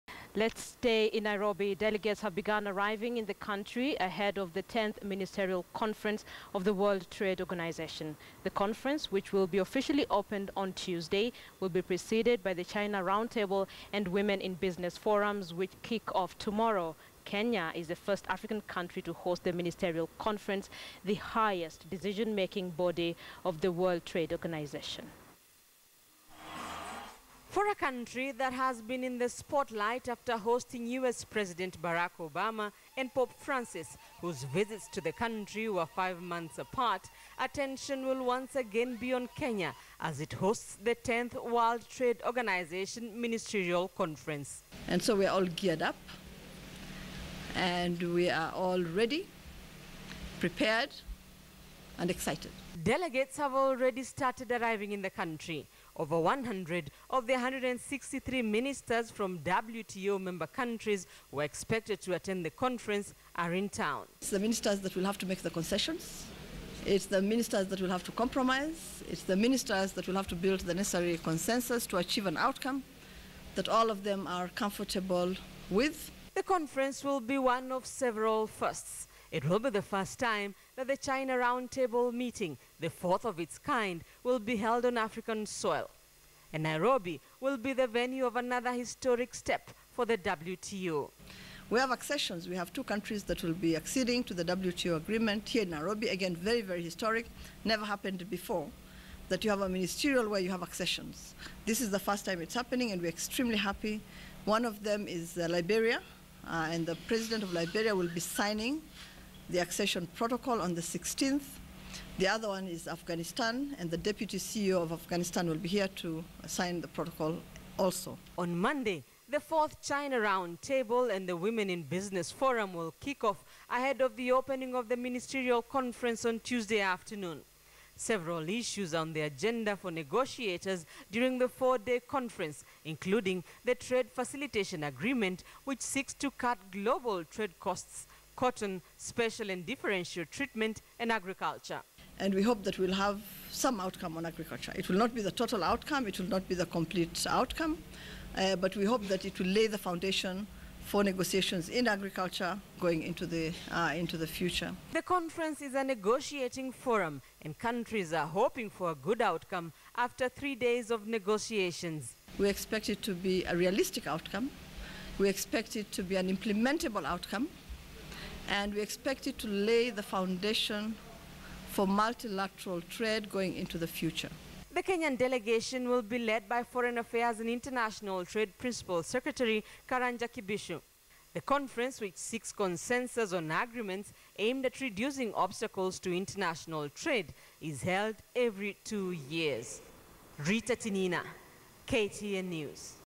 WTO CONFERENCE; Kenyan farmers speak of expectations
Delegates have begun arriving in the country ahead of the 10th ministerial conference of the world trade organization. The conference which will be officially opened on Tuesday will be preceded by the china roundtable and women in business forums which kick off tomorrow. Kenya is the first African country to host the ministerial conference, the highest decision making body of the WTO.